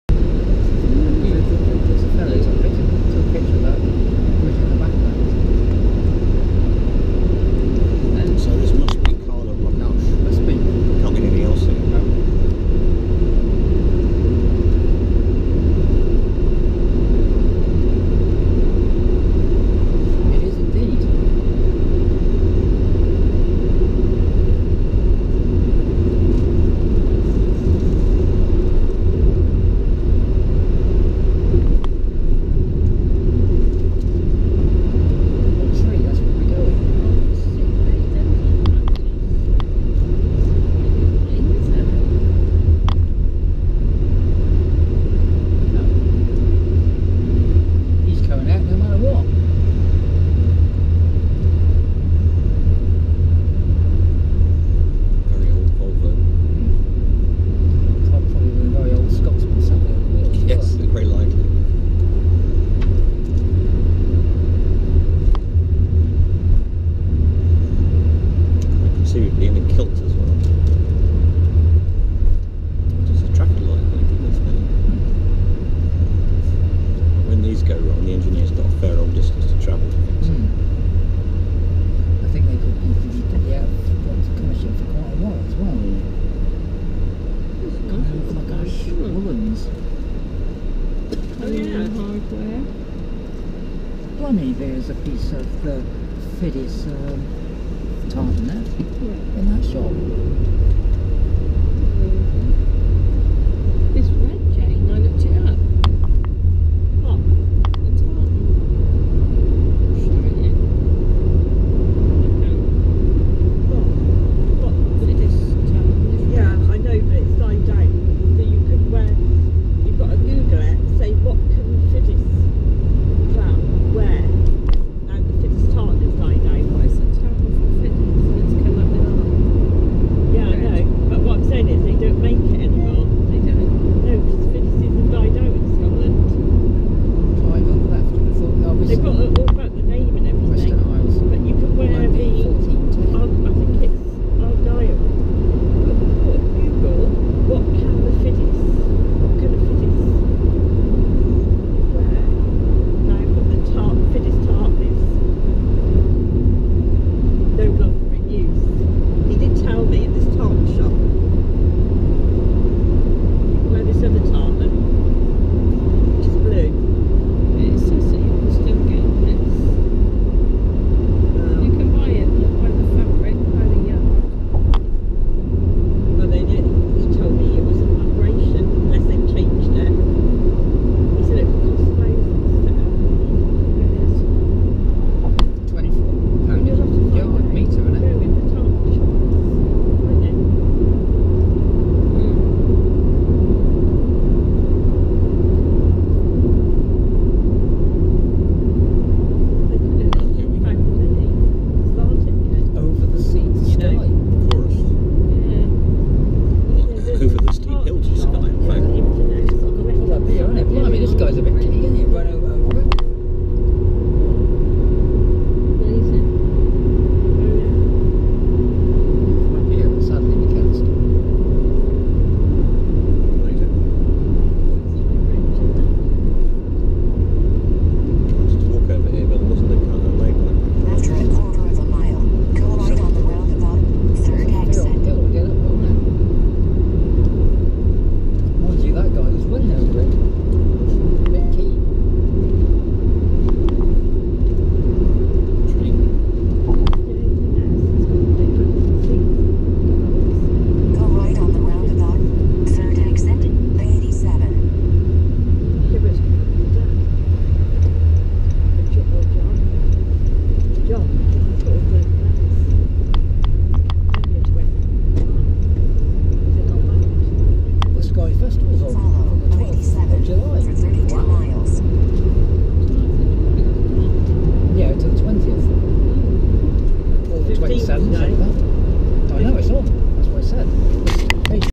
Scotland by Road - Skye Bridge and Kyle of Lochalsh
In this video, part of the Scotland by Road series, we enter Kyle of Lochalsh and then pass through it onto and over the Skye Bridge